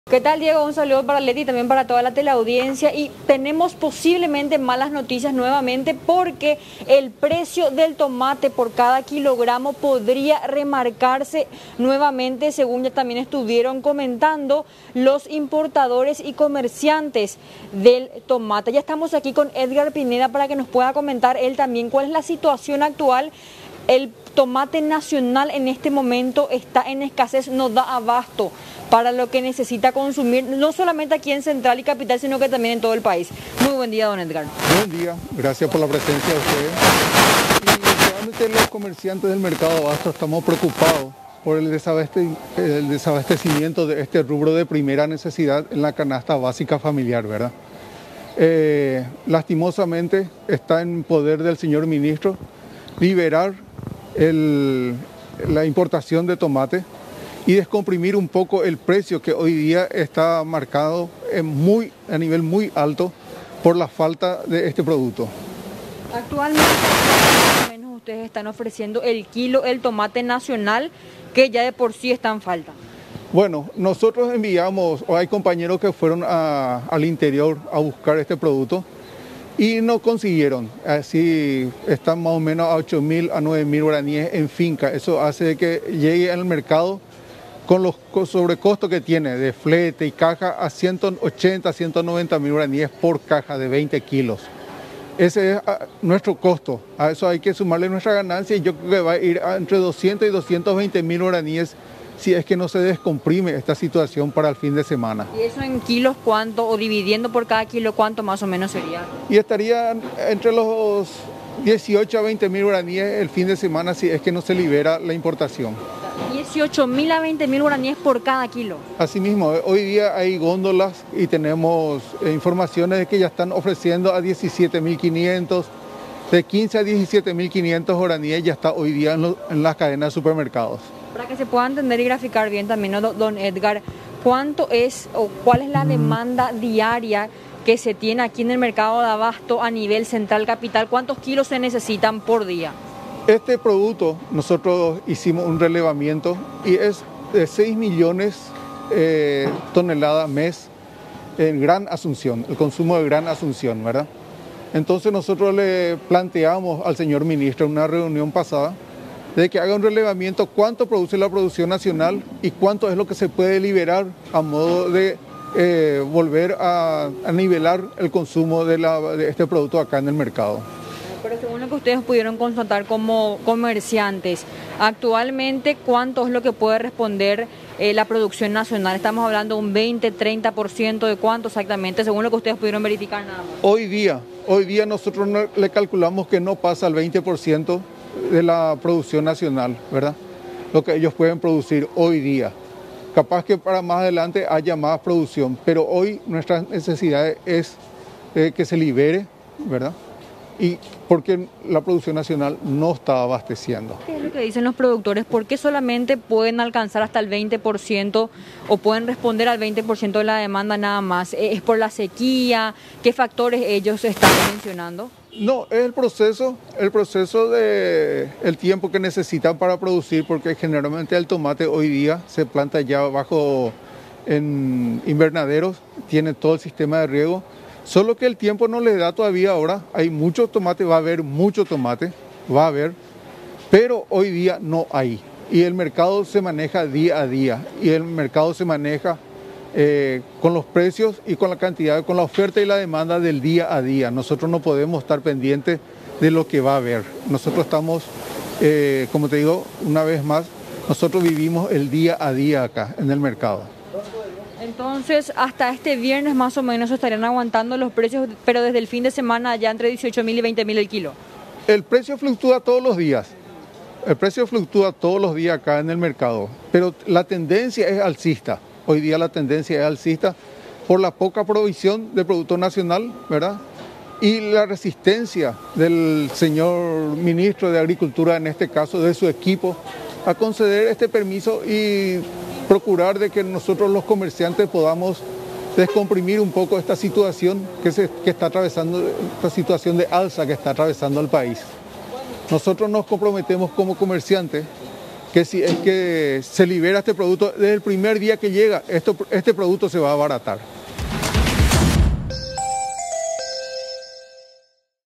Precio del tomate podría subir aún más
El precio del tomate por kilo podría remarcarse y volver a aumentar, según los importadores y comerciantes. El tomate nacional está en escasez y eso produciría un nuevo reajuste, llegando incluso a los G. 20.000 por kilo. Según afirman, esto se subsanaría liberando la importación de la fruta.